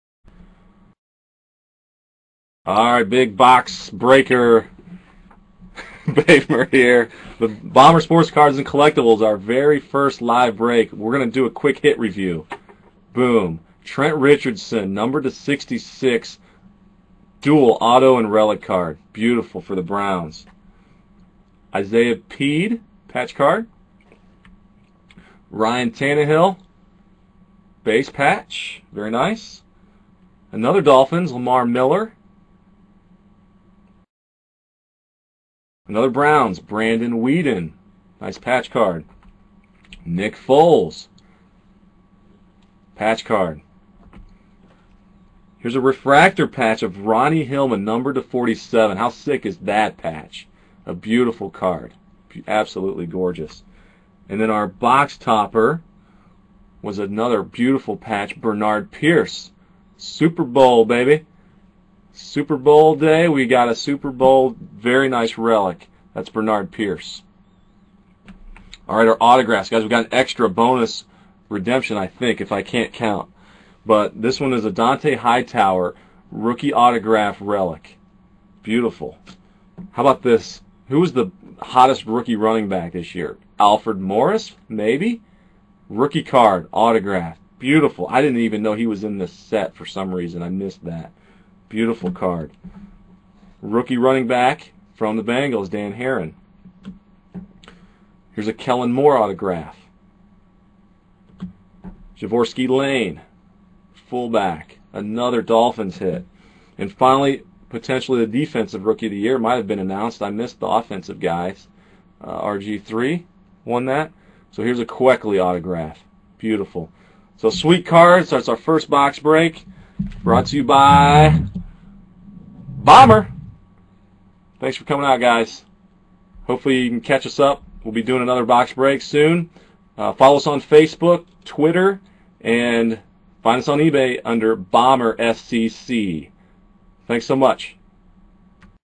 1st Big$Hit Live Break "Just The Hits" Super Bowl Sunday
Richardson, Morris, Hillman, Hightower and Kuechly Highlight a very nice 2012 Bowman Sterling Football Box.